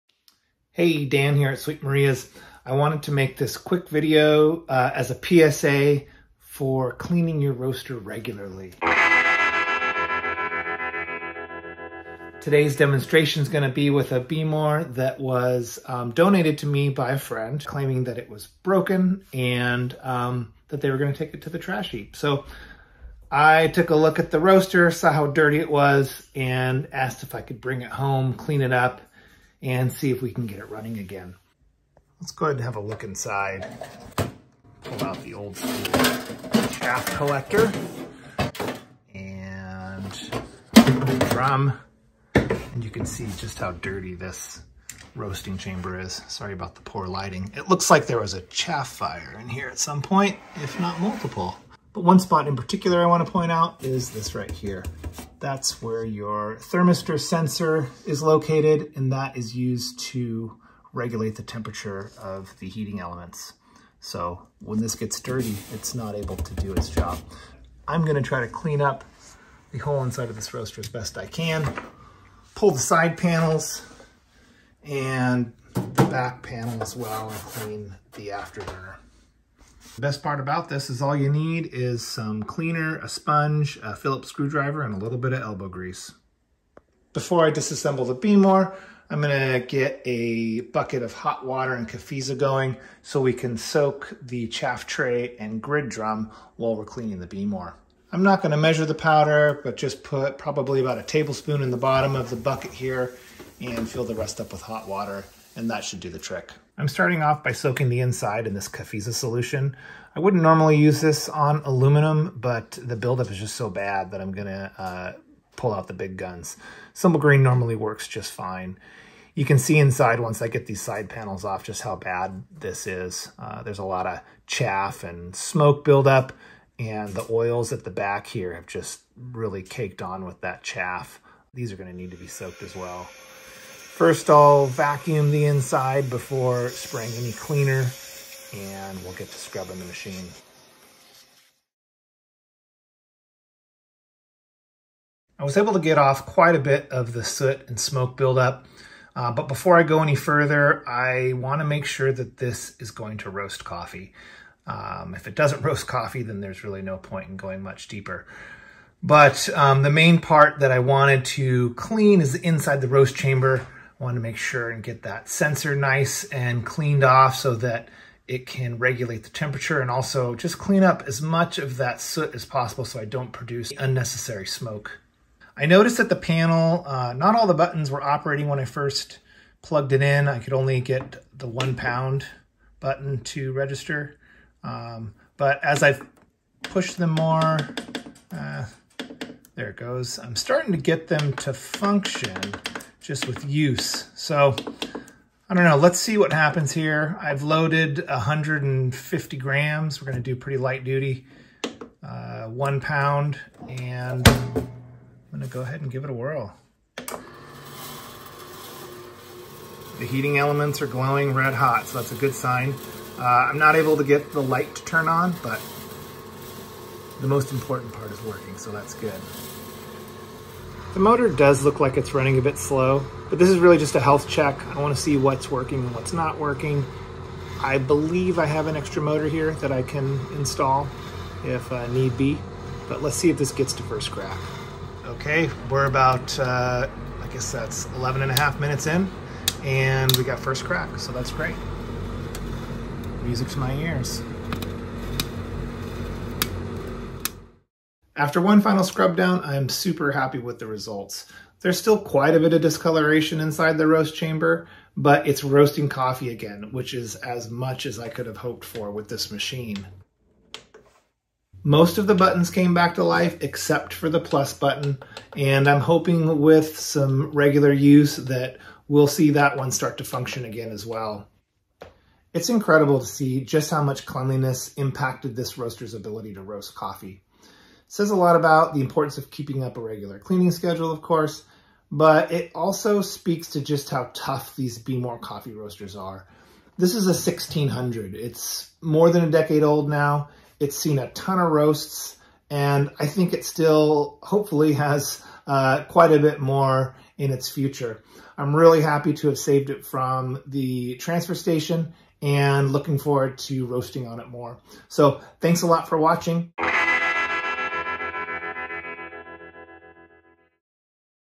Reviving a Behmor Coffee Roaster: Behmor's Are Built to Last
Just how tough are Behmor coffee roasters? How about we let this poorly maintained Behmor 1600 (first generation) answer that question.

This Behmor was recently abandoned as "broken", unable to reach 1st Crack no matter how much time was added to the roast cycle. A quick inspection of the filthy interior told a much different story. Caked on smoke and oil residue looked to be a possible source of the problem, and I was pretty sure a good cleaning would go a very long way given the Behmor's impressive resiliency to this type of abuse.

Check out this roaster's journey to getting a new lease on life.